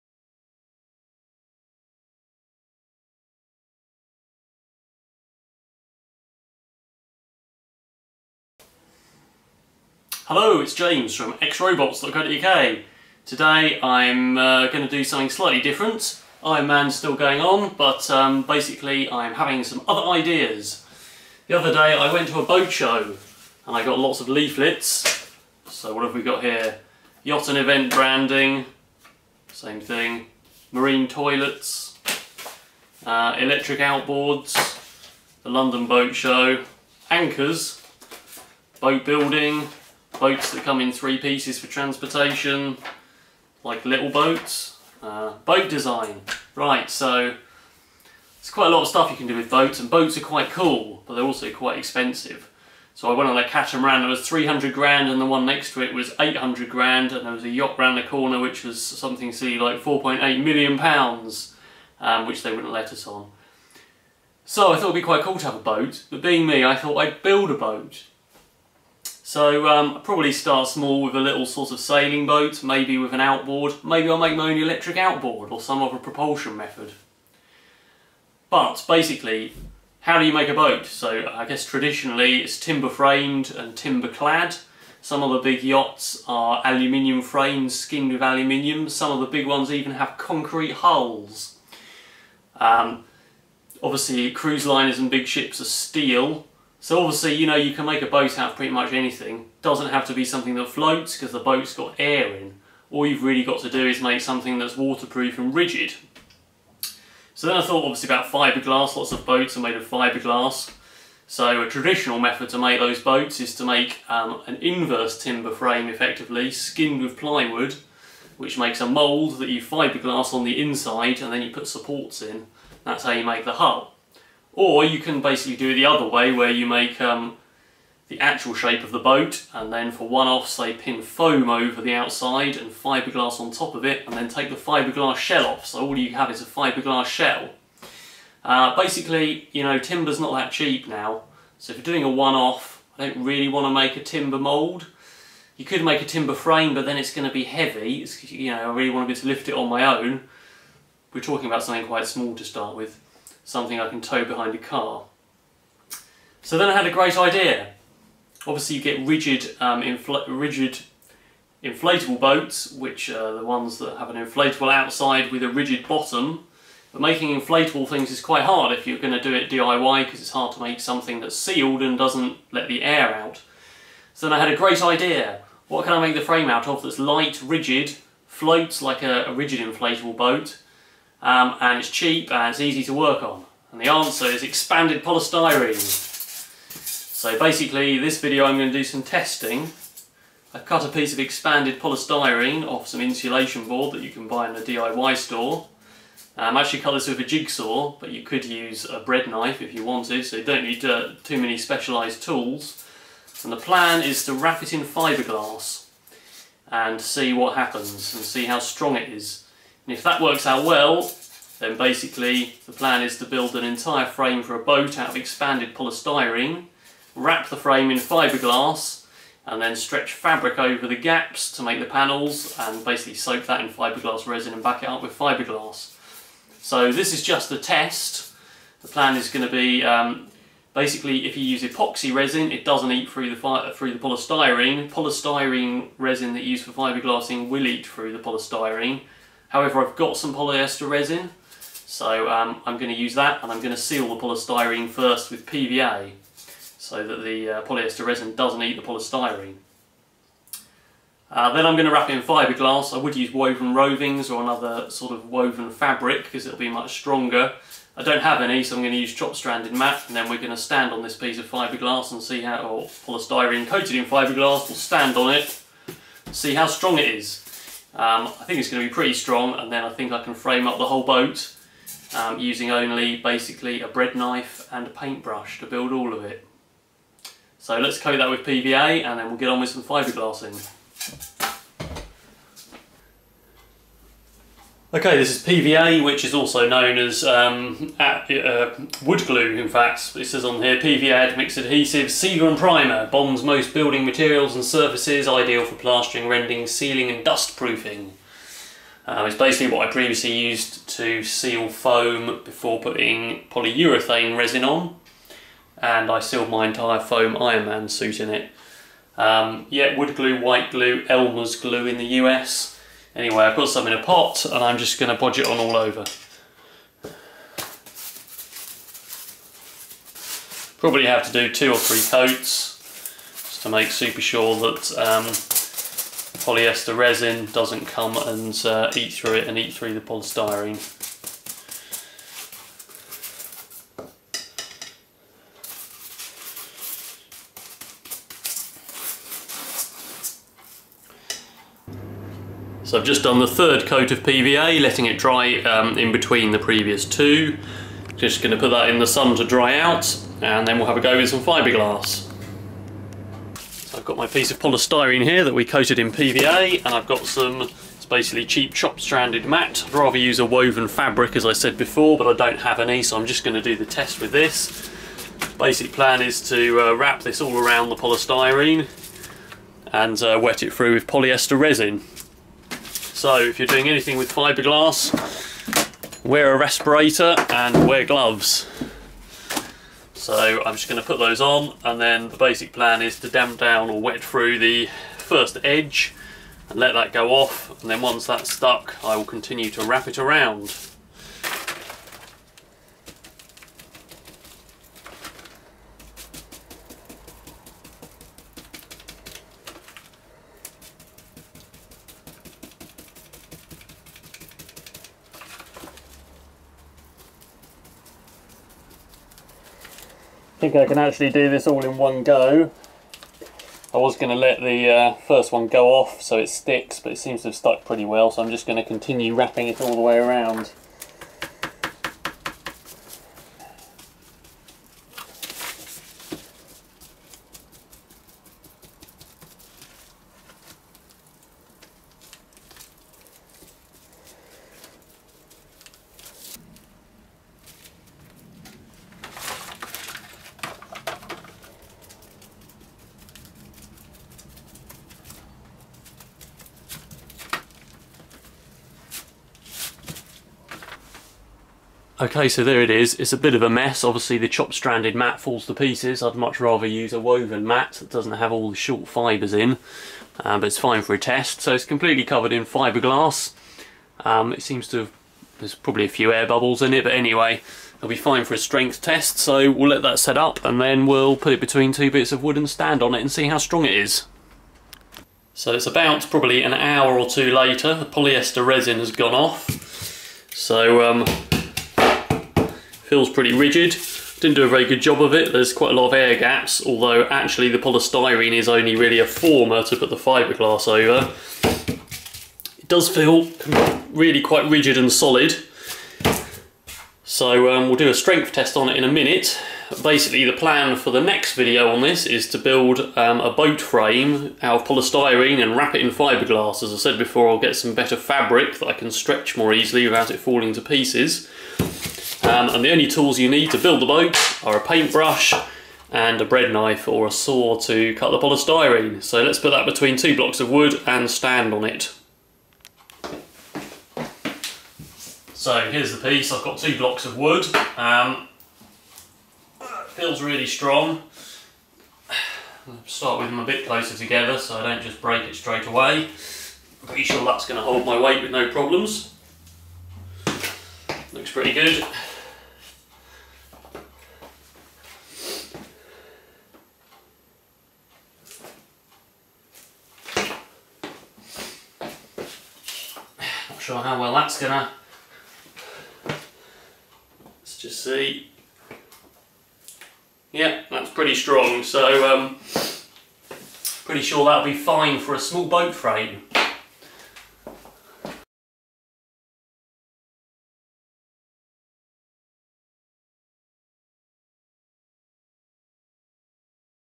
Experimental Boat Building Part 1, testing expanded polystyrene and fiberglass composite
This is the first part of my experimental boat building series, more